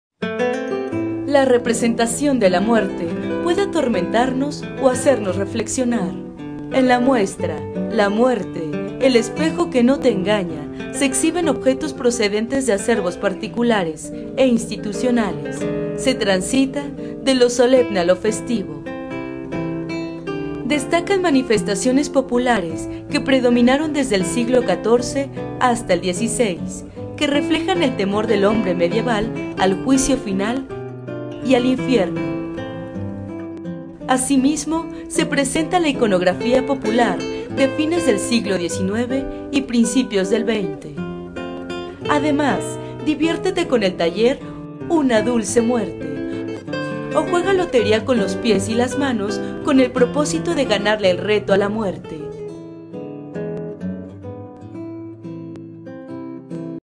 La Muerte el Espejo no te Engaña
De lo solemne a lo festivo transita el espectador en la exposición La muerte. El espejo no te engaña, montada en el Museo Nacional de Arte hasta el 30 de noviembre.

Está compuesta de 88 piezas entre textil, loterías, hojas volantes, libros, reproducciones en impresión tipográfica, revistas, gacetas, dibujos, fotografías, portadas de revistas o cuentos y otros impresos.